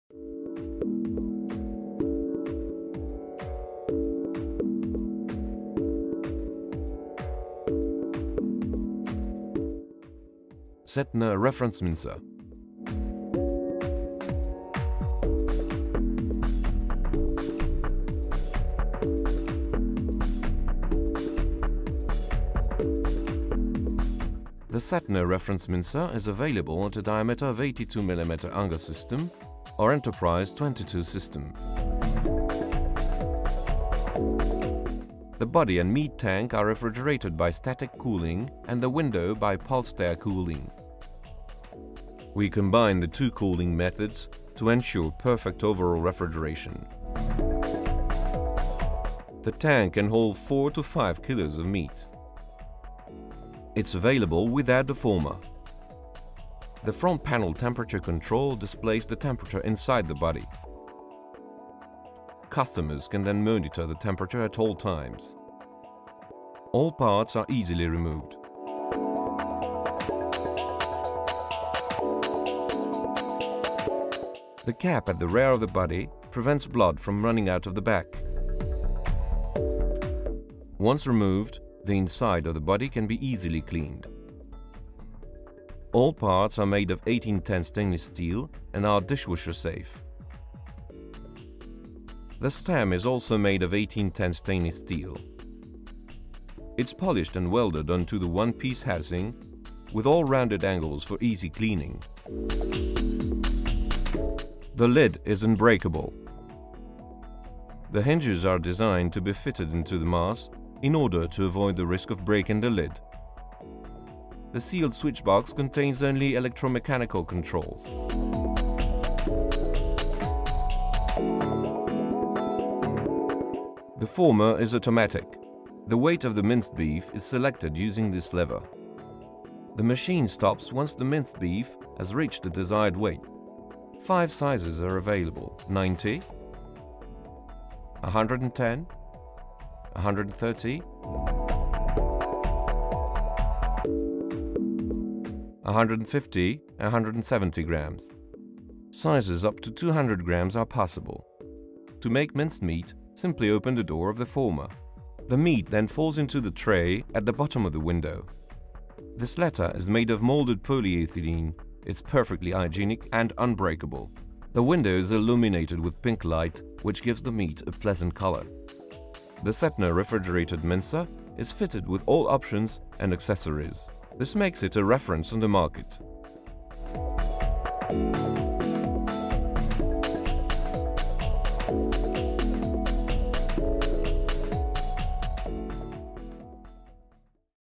REFRIGERATED MEAT MINCER SETNA GB
- TRI 400V, 50Hz
- System unger
- With portionner
- Entirely stainless steel 18-10
- Monobloc carter and dismountable body
- Cooled body and storage place
- Fine glossy stainless steel finish
- Red light in the window
- Refrigeration by hermetic compressor and static and pulsed cold
- Regulation by digital display electronic thermostat - Gaz: R600a
- Portionner with 5 automatic calibers: 90 / 110 / 130 /150 / 170 grams
- Motor 1,5 CV (1,1Kw)
- Theorical output 300 Kg/h
- Hopper capacity 5L
- Exit diameter 82mm
- Grate diameter 3mm
- Net weight 70Kg
- Dimensions 513x565xH621mm
- Complies with CE norms
- Made in France